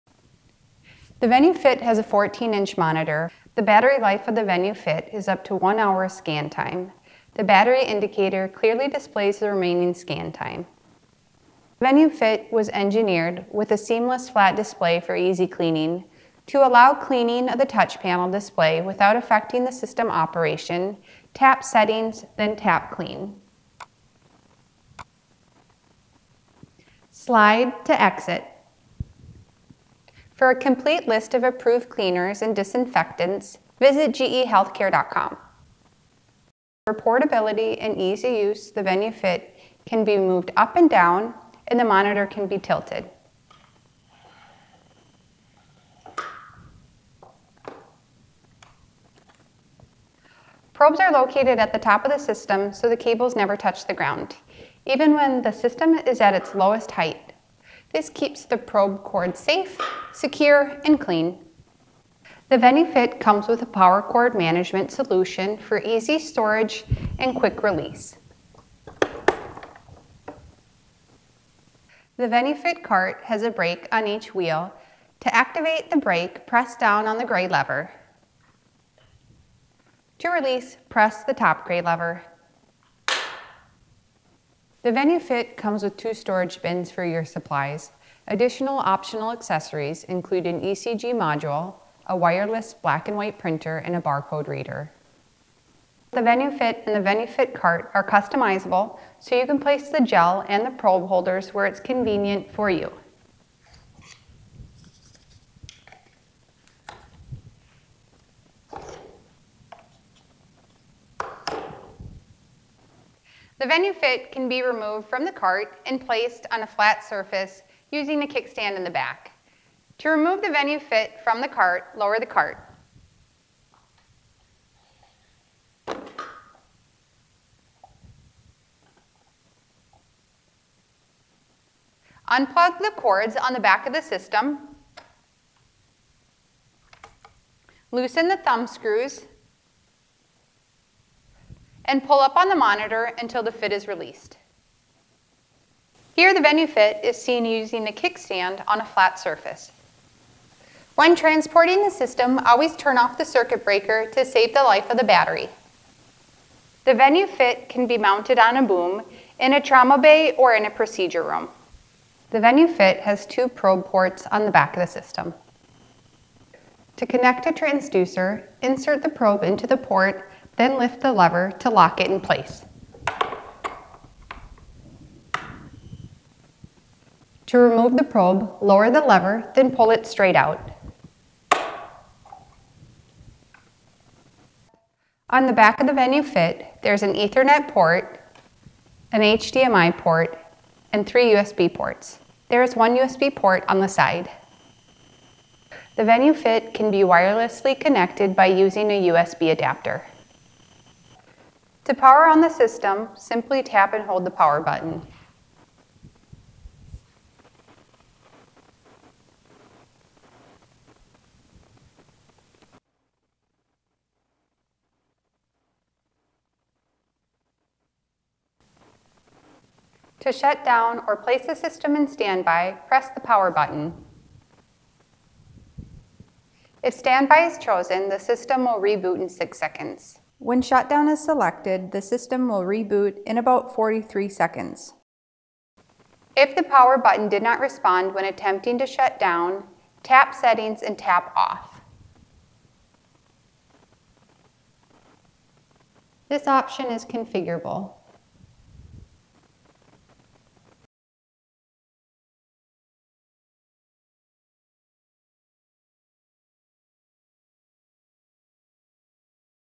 Venue Fit™ Tutorial Video 2021
Learn about Venue Fit™, GE Healthcare's point of care ultrasound system.